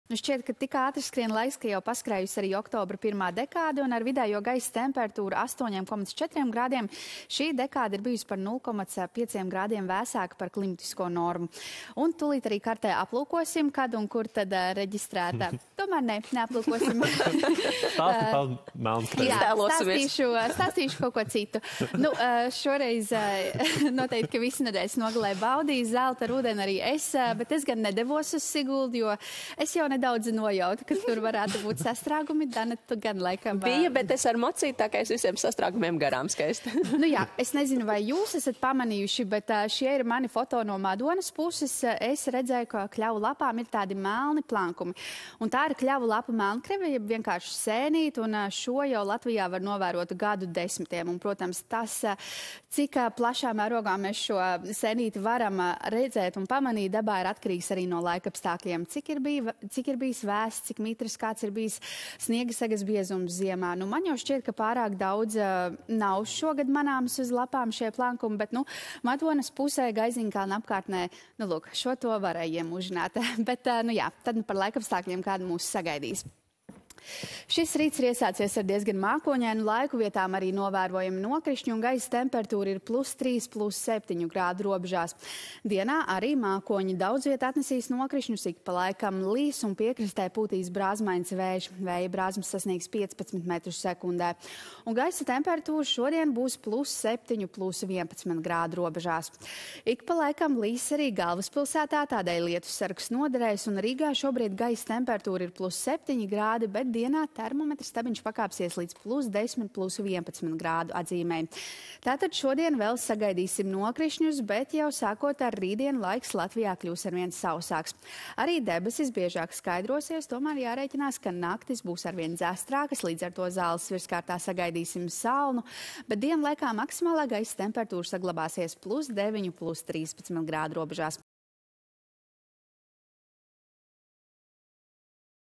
15. oktobra laika ziņas
Otrdien mākoņi brīžiem izklīdīs, tomēr valsts lielākajā daļā palaikam gaidāms lietus. Dienā pūtīs lēns līdz mērens ziemeļrietumu, ziemeļu vējš, kas piekrastē brāzmās sasniegs 15 m/s. Maksimālā gaisa temperatūra būs +8..+11 grādu robežās.